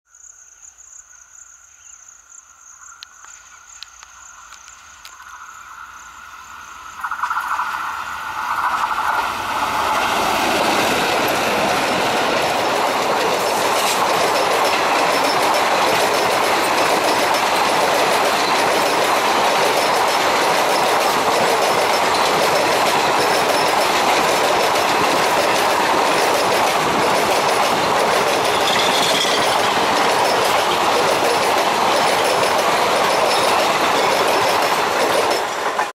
ET WDM3D TWINS ALCO 19046 19046 Chhapra - Surat Tapti Ganga Express : Indian Railways
ET WDM3D TWINS ALCO WITH 19046 CHHAPRA JUNCTION - SURAT TAPTI GANGA EXPRESS SKIPPING BAGRA TAWA STATION OF WEST CENTRAL RAILWAYS.